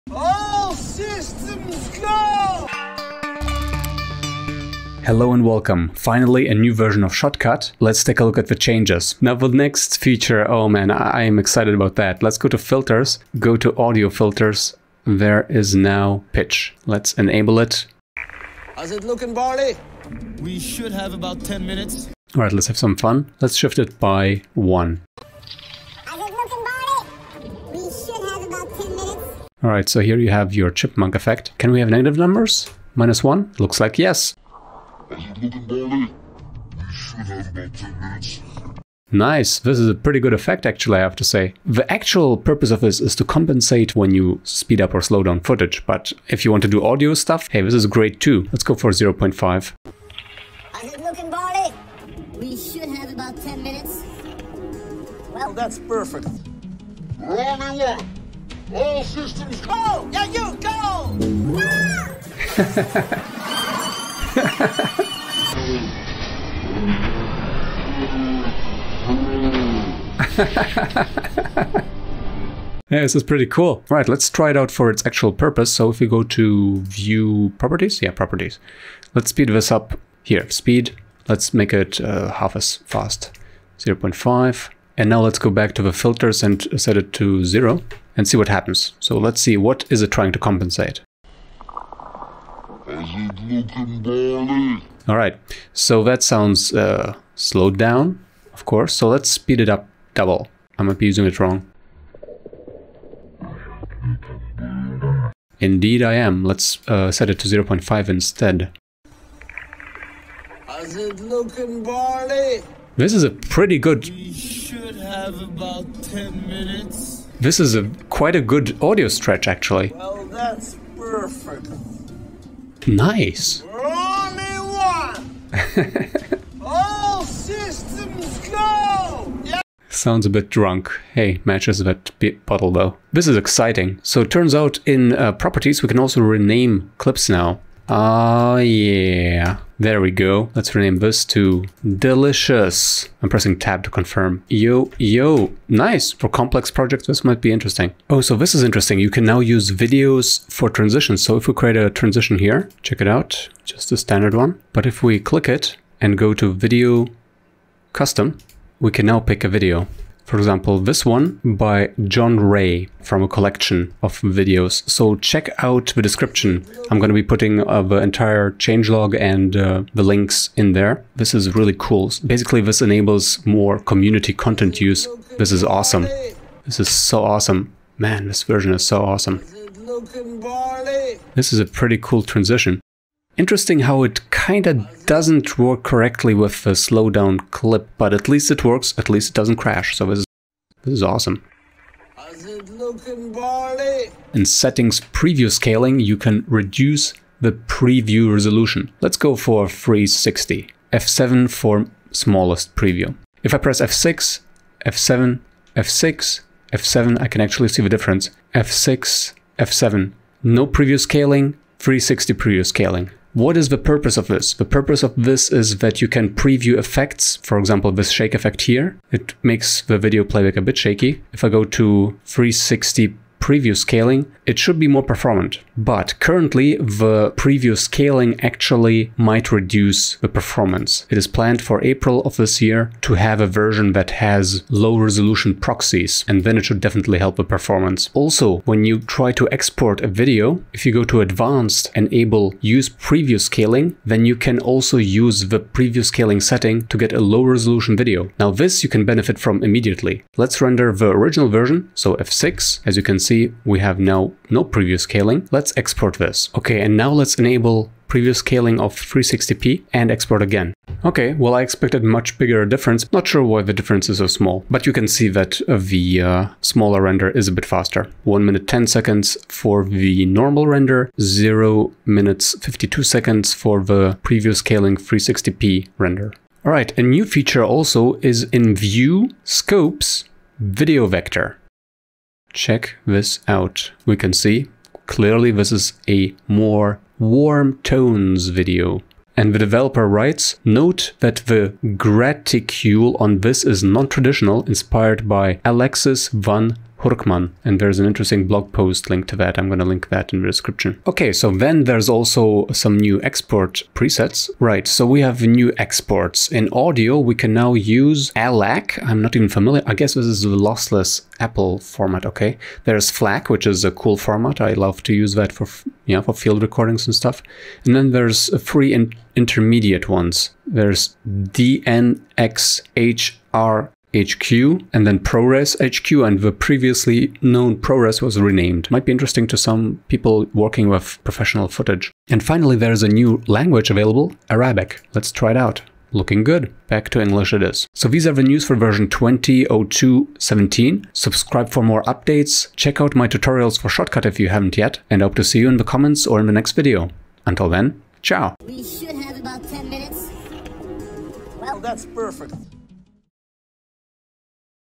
New Features in the Free Video Editor Shotcut!
💬 Write a comment with more info, questions or tutorial requests!

- Added Settings - Preview Scaling!
This reduces the resolution for rendering effects for preview within the app, but there may be added a fair cost to the CPU for down-scaling sources to the preview resolution. Thus, this will be more beneficial when low resolution proxies are created, which is planned for version 20.04 in April.
Preview scaling will not always provide perfect fidelity. Some differences are unavoidable especially where blur, softness, and noise are concerned. Some filters may not support preview scaling and will process at full project resolution. (D'oh - stabilize is one of them - the one I was showcasing! =[)
- Added Export - Advanced - Video - Use preview scaling.
This is intended to provide a faster export for draft review. You should leave the export resolution set to the project resolution unless your preset or video codec dictates a resolution.
- Added Views - Scopes - Video Vector.
Note that the graticule on this is non-traditional, inspired by Alexis Van Hurkman.
- Added Filters - Audio - Pitch.
This is handy when changing the speed of a clip anywhere from 4.0x - 0.25x. AND FOR PLAYING AROUND WITH FUNNY VOICES :D
- Added the ability to rename clips in Properties by clicking into the header where the name is displayed.
- Added support for using a video clip in Transition Properties - Video.
- Added a few more export presets:
audio/ALAC
audio/FLAC
intermediate/DNxHR HQ
intermediate/ProRes HQ
intermediate/ProRes was changed to ProRes 422
- Added Arabic translation.

Shotcut is a free and open source video editor with many built-in filters (effects). It is a small download (less than 100 MB) and actively developed.